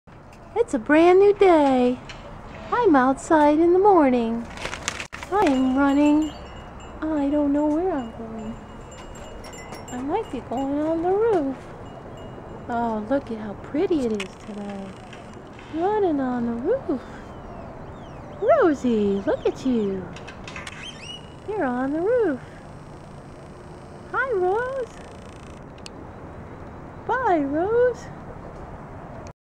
A New Day for Rosie Posie
Rosie demonstrates that little squirrels are a font of limitless energy!
A good night's sleep and off she goes again!  Have fun Rosie!
(And BE SAFE!)